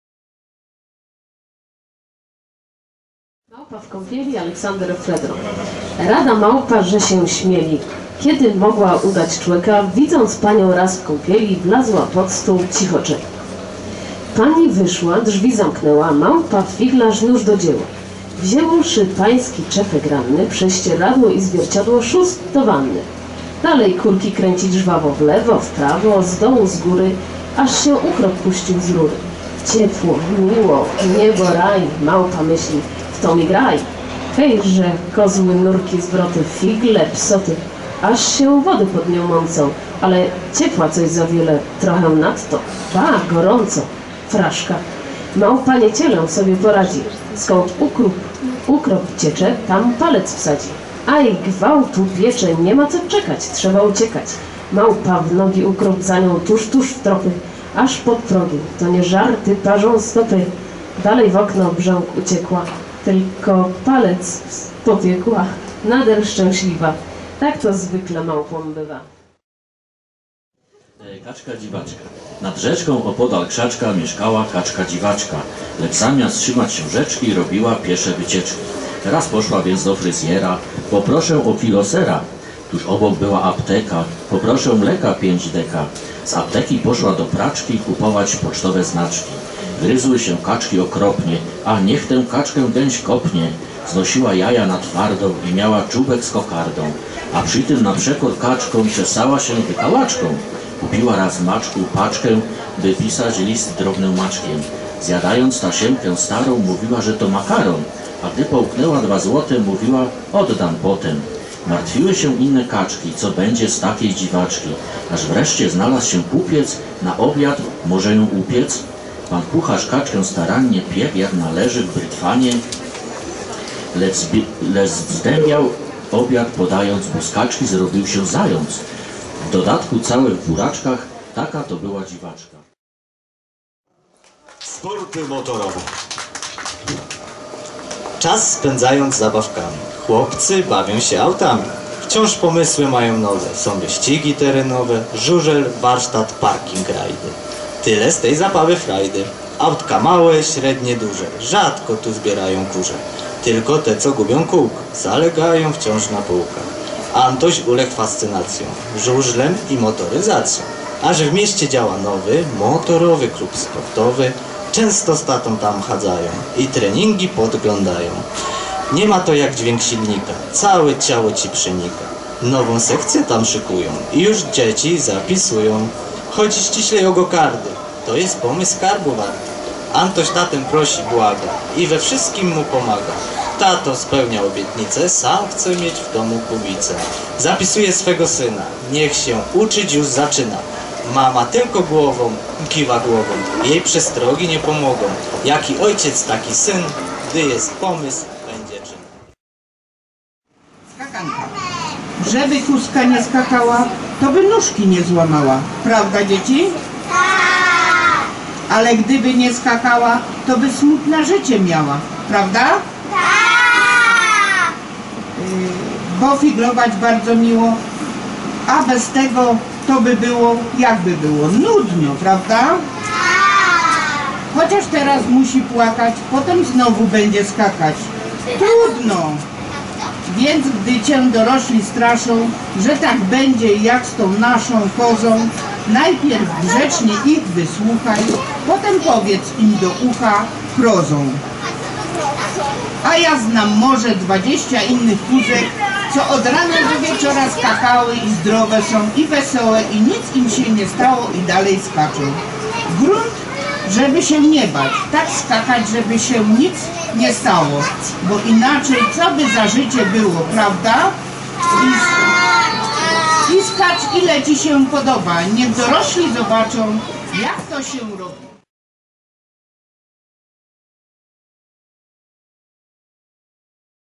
Sportowcy czytają dzieciom
Filmik oficjalnie otwierający projekt aniBAJKI. Mistrzowie sportowi czytali dzieciom podczas happeningu Biblioteki Miejskiej w Cieszynie i Radia 90.
Czytali dla nas: snowboardzistka Paulina Ligocka, lekkoatleta Janusz Rokicki oraz armwrestlerzy Maria Juroszek i Robert Hubczyk. Dziękujemy!